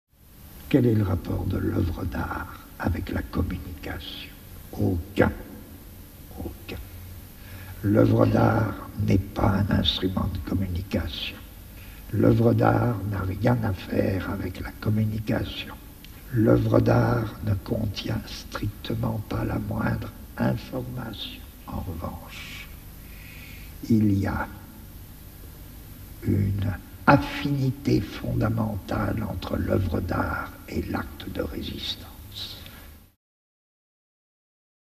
L'art et la communication
L'art et la communication | L'art et les sociétés de contrôle | G.Deleuze | INA archives FR